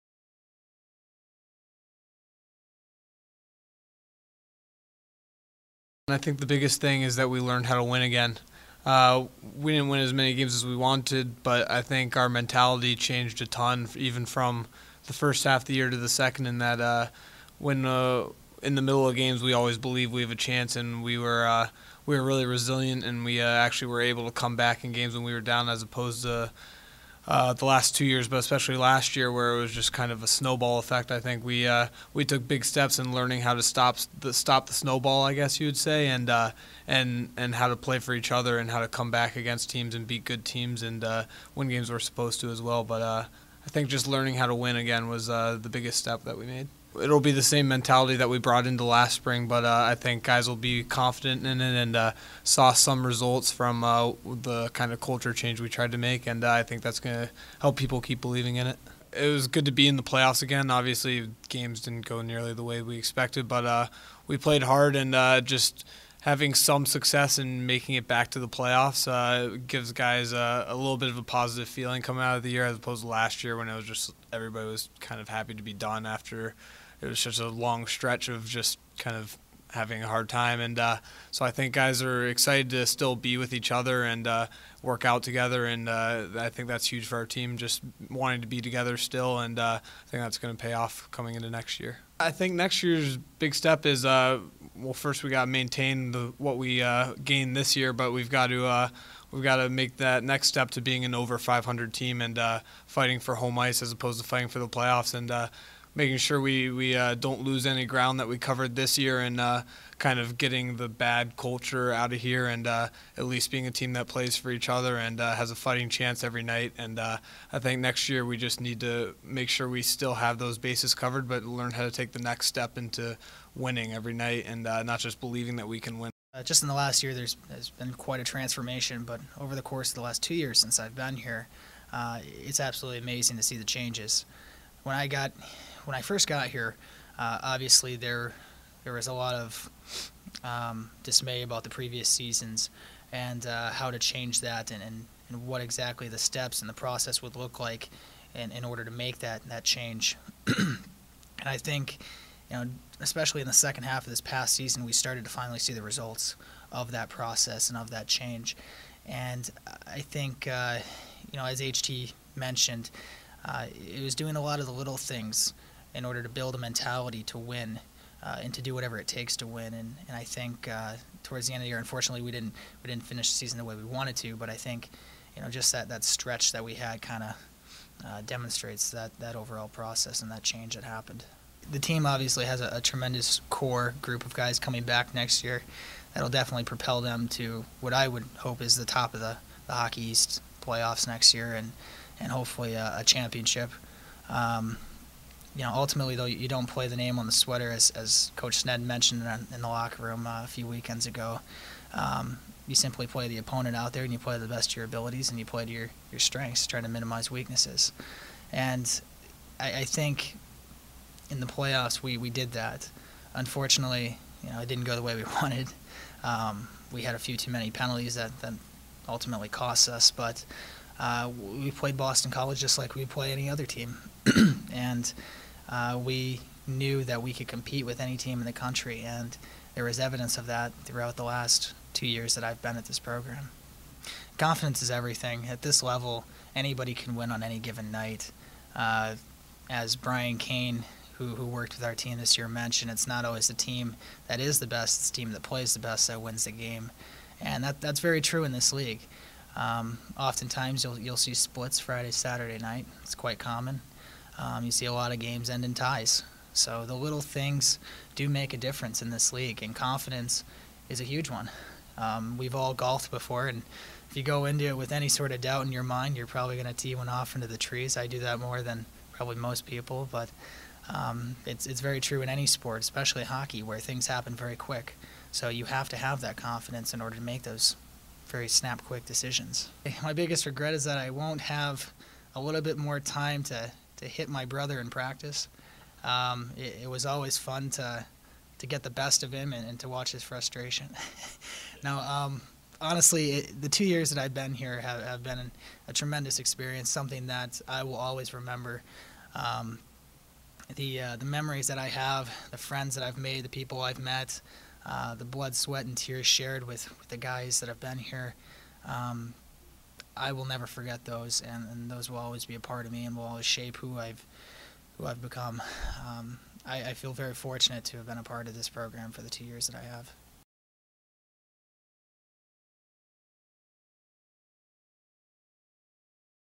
Men's Hockey: End of Season Press Conference, Players (3/21/13)
Vermont forwards H.T. Lenz and Brett Bruneteau speak with the media following the end of the 2012-13 hockey season. The Catamounts finish with a record of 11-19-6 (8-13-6 Hockey East).